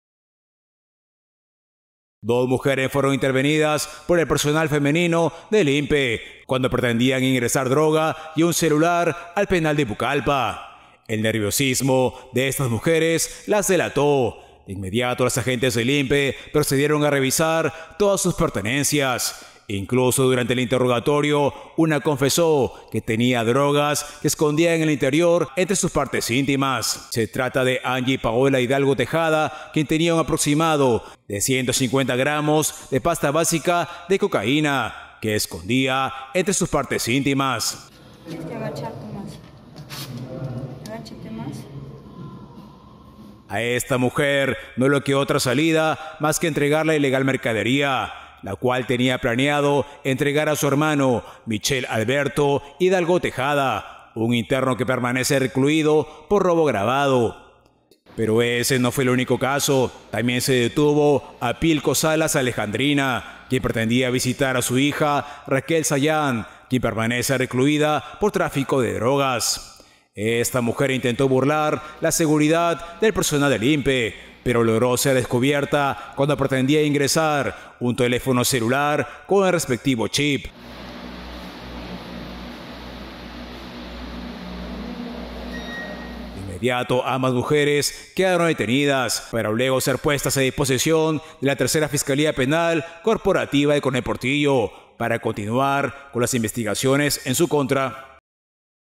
DOS MUJERES SON DETENIDAS CUANDO PRETENDÍAN INGRESAR DROGAS Y UN CELULAR AL PENAL DE PUCALLPA
EN EL PENAL DE PUCALLPA, DOS MUJERES QUEDARON DETENIDAS LUEGO DE INTENTAR INGRESAR MÁS DE CIEN GRAMOS DE DROGAS Y UN TELÉFONO CELULAR. ELLAS PRETENDIAN ENTREGAR ESTOS PRODUCTOS PROHIBIDOS A SUS FAMILIARES INTERNOS